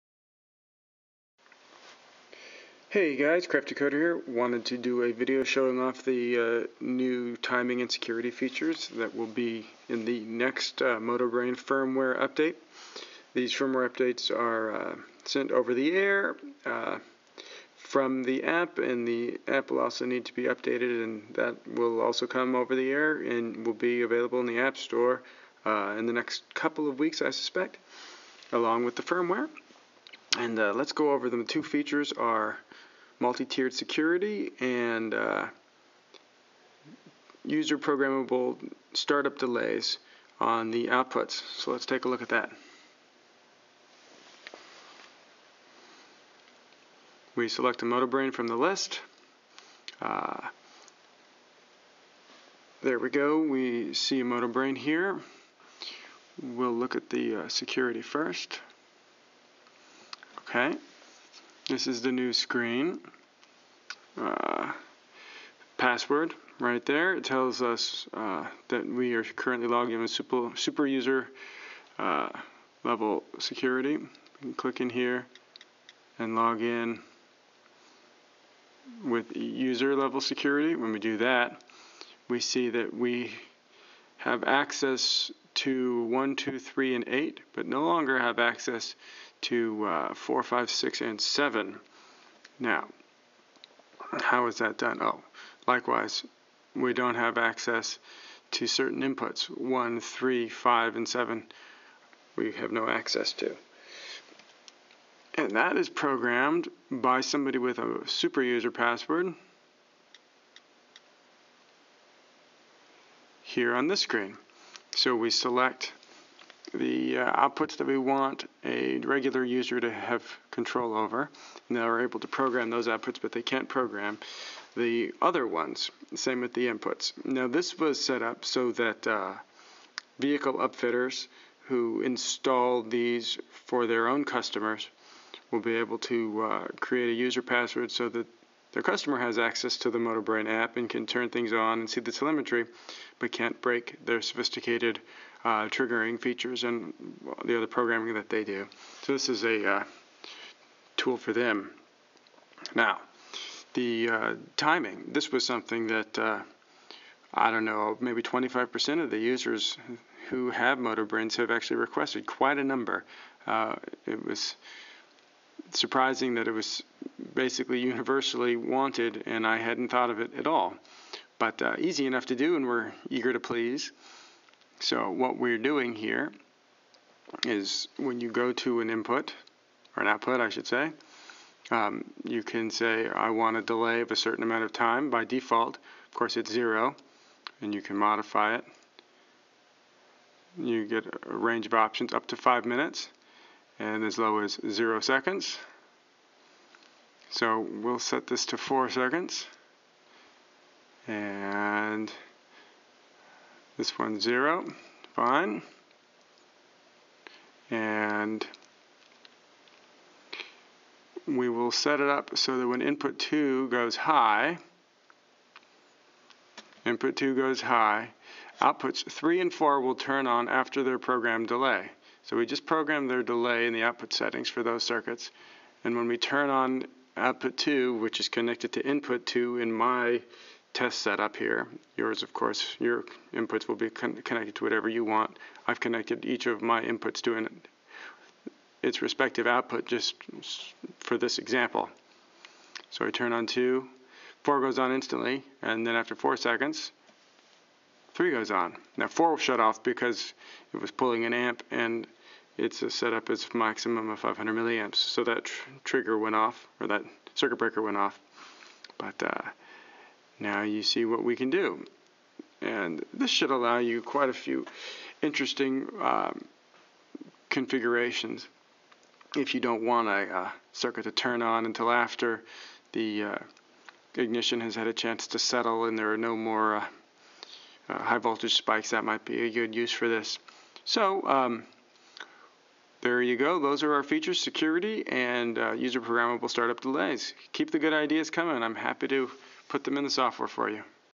Motobrain: New Security and Timer Features Explained
These new features will be released in an over-the-air firmware and software update in the new couple of weeks. These ideas came direct from users of the product and they are released with my thanks.